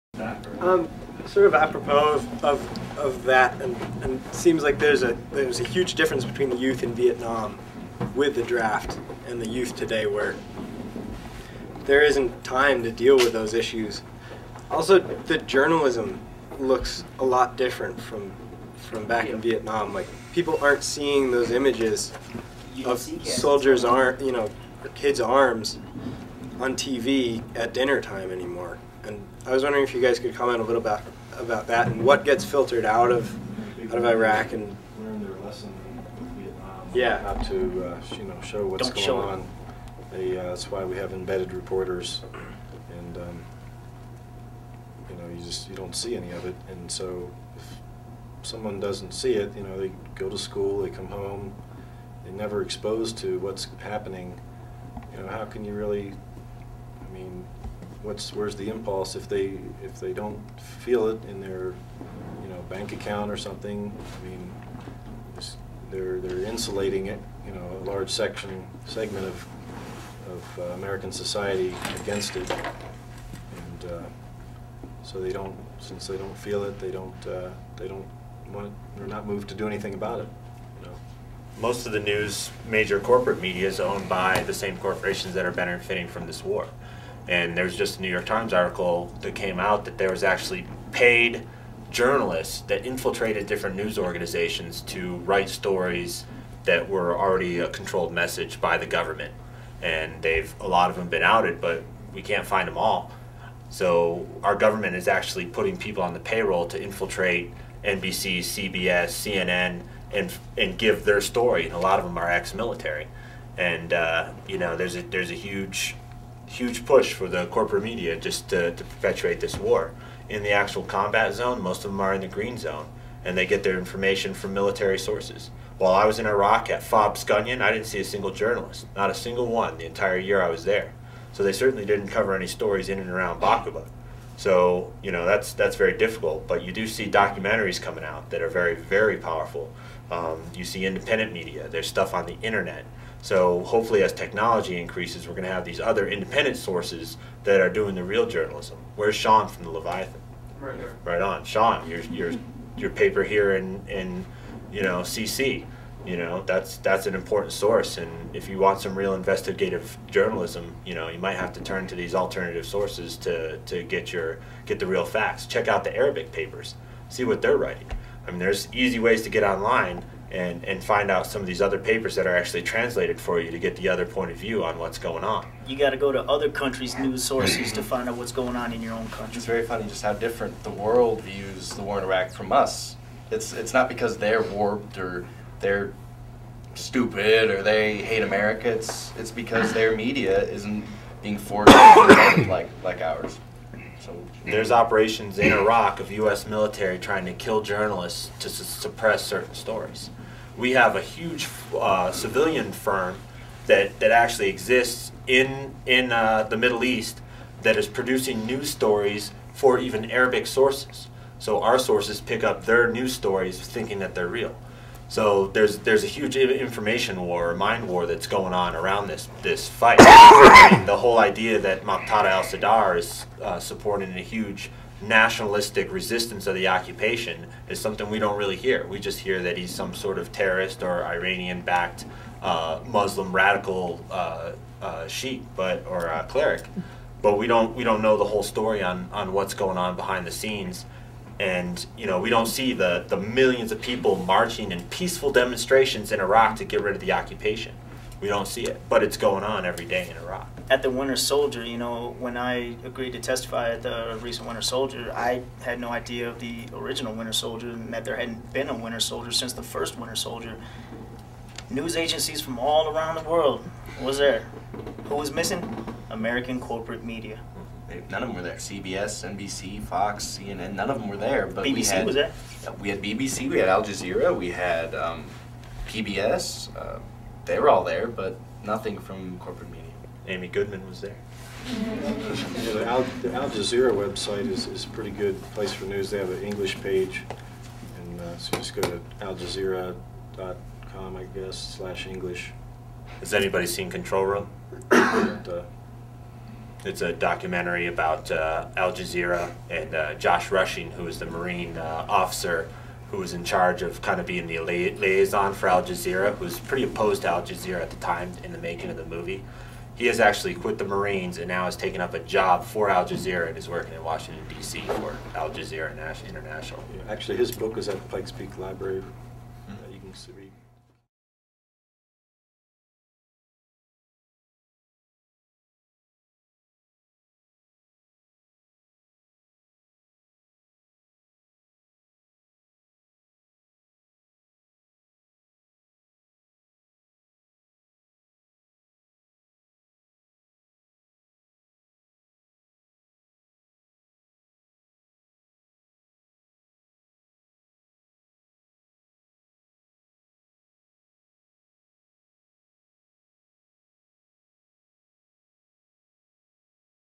IVAW Winter Soldier CS Q8 Media
Question 8 on media coverage of Iraq compared to Vietnam.

This is the Winter Soldier Colorado Springs hearings: testimony from U.S. veterans who served in the Iraq and Afghanistan occupations, giving an accurate account of what is really happening day in and day out, on the ground.

This series took place at Colorado College on Tuesday, April 22, in the Max Kade Theater, after showing part of the original Winter Soldier Hearings from Detroit in 1971.

Veterans appearing are Garett, Mark, Brian, and Dom.